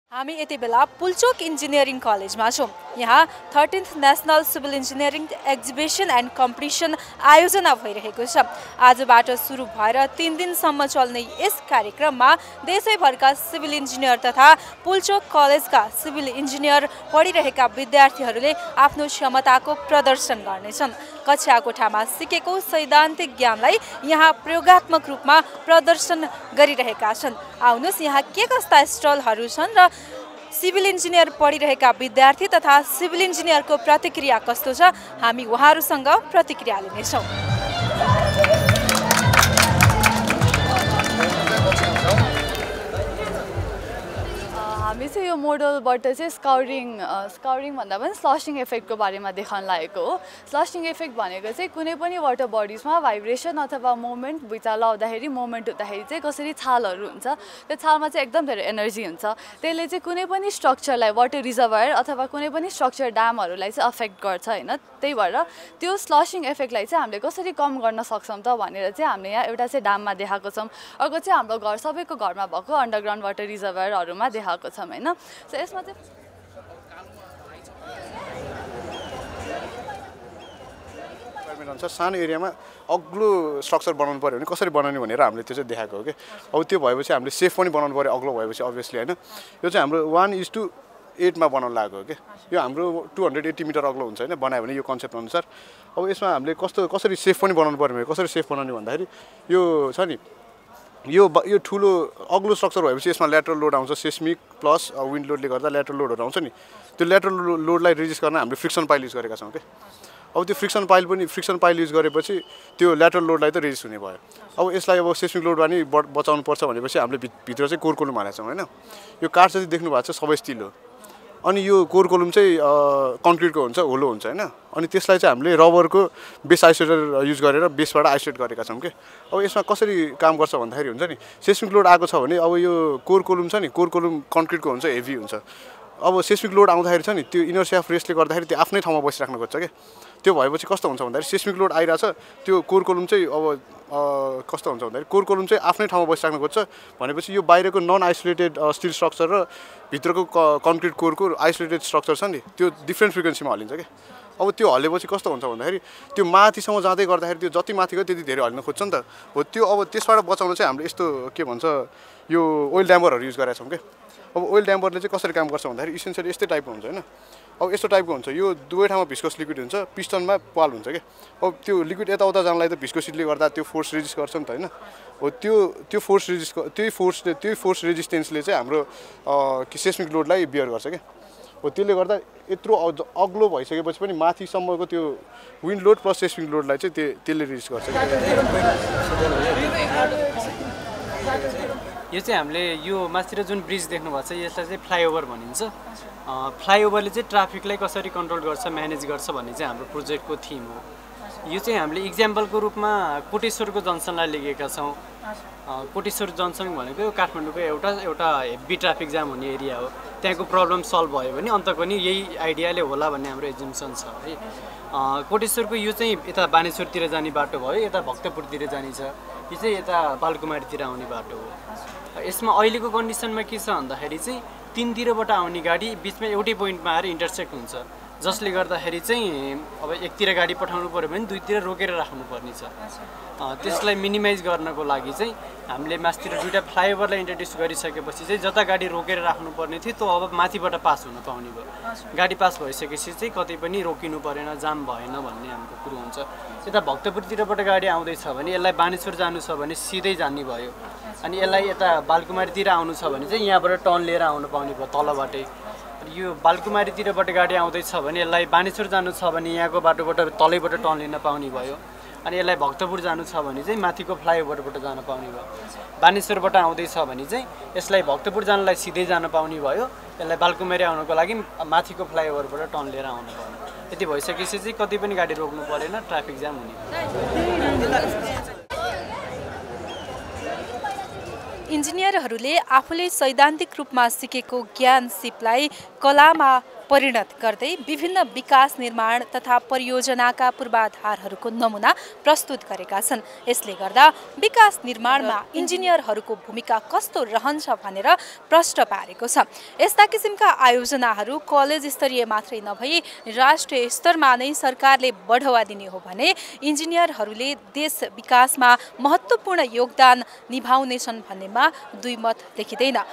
Pulchowk Engineering Collegeमा १३ औँ सिभिल इन्जिनियरिङ प्रदर्शनी तथा प्रतिस्पर्धा सुरु
Pulchowk Engineering Collegeमा १३ औँ सिभिल इन्जिनियरिङ प्रदर्शनी तथा प्रतिस्पर्धा आजबाट सुरु, के कस्ता छन् प्रदर्शनी?

Presenter: Prakriti Bhat
Camera: Pujan Pokharel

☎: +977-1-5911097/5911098